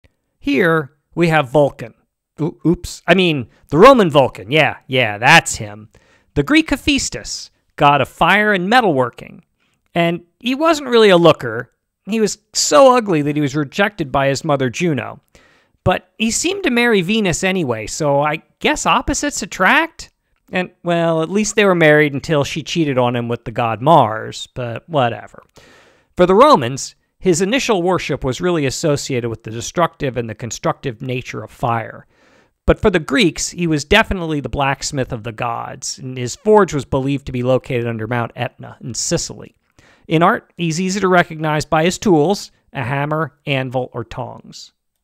Vulcan, Hephaestus: God of fire and hammering
For the Romans, his initial worship was associated with the destructive and constructive nature of fire.

For the Greeks, he was the blacksmith to the gods, and his forge was believed to be located under mount Etna in Sicily.

In art you can recognize him by his tools, a hammer, anvil or tongs.